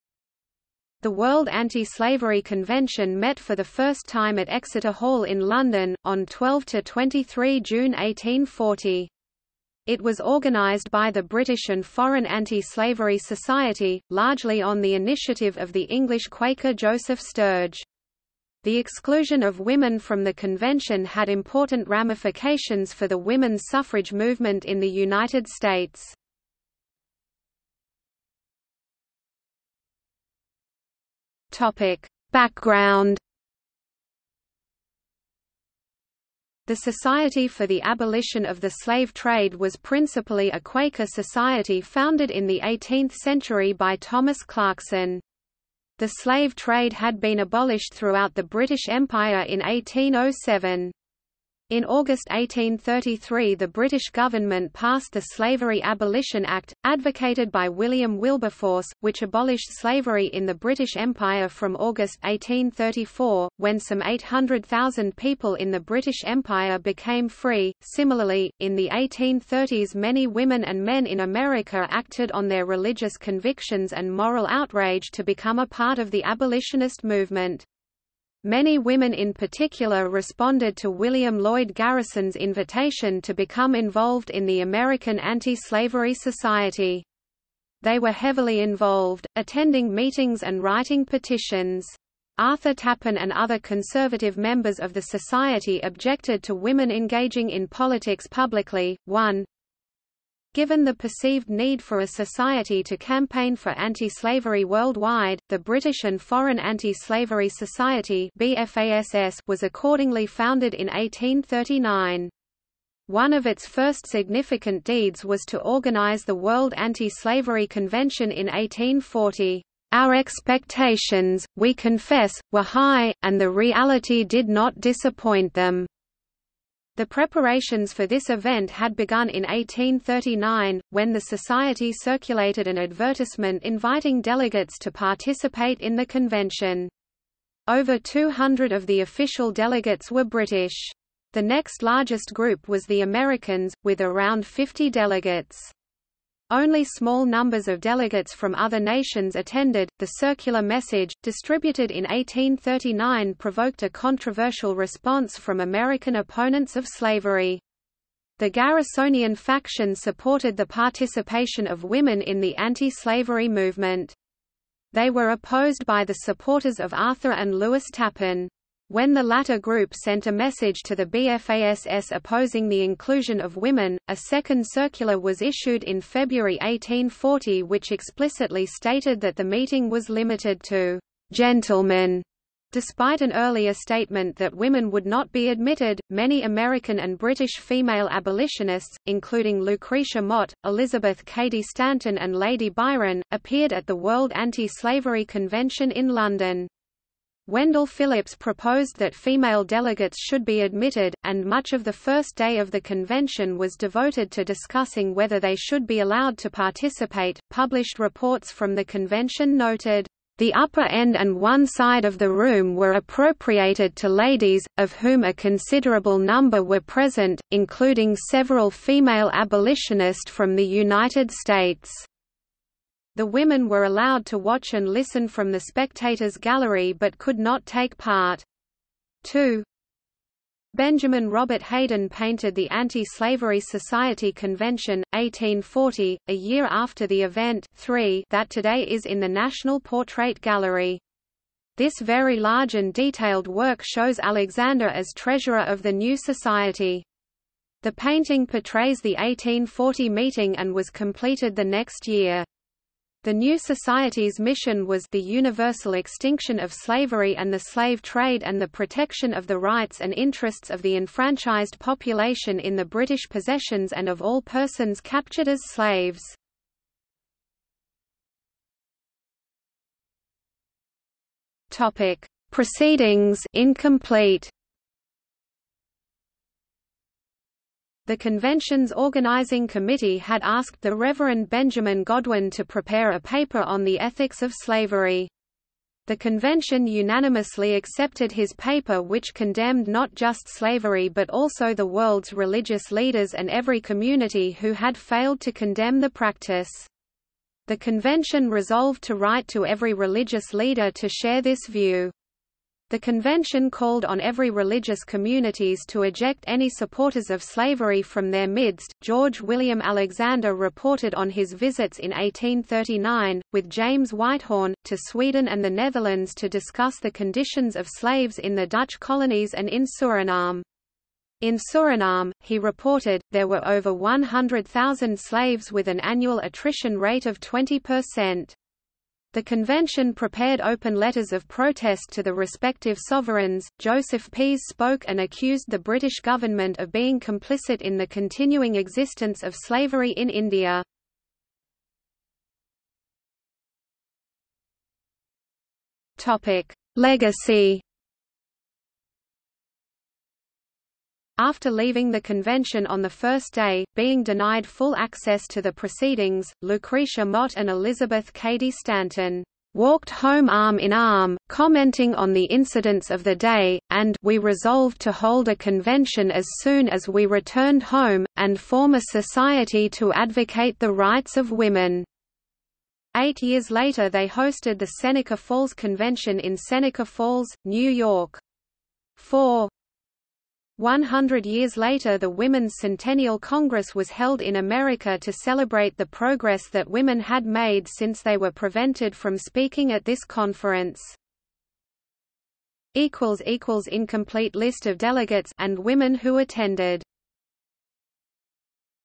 World's Anti-Slavery Convention | Wikipedia audio article
00:00:28 1 Background
00:04:28 2 Proceedings (incomplete)
00:05:42 3 Legacy

- Socrates

SUMMARY
The World Anti-Slavery Convention met for the first time at Exeter Hall  in London, on 12–23 June 1840. It was organised by the British and Foreign Anti-Slavery Society, largely on the initiative of the English Quaker Joseph Sturge. The exclusion of women from the convention had important ramifications for the women's suffrage movement in the United States.